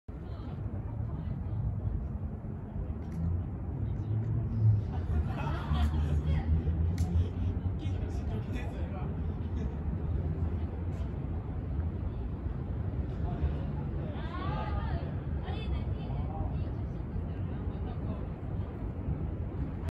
東京スカイツリー ライトアップ 雅 2022 Asakusa Sumida Park 浅草 隅田公園 大寒桜 桜 さくら ☆ 満開！ 3月20日(日) 夜桜 遊具広場前
2022年　3月20日(日) 　IMG 9130

22:00～24:00は「雅」を点灯します。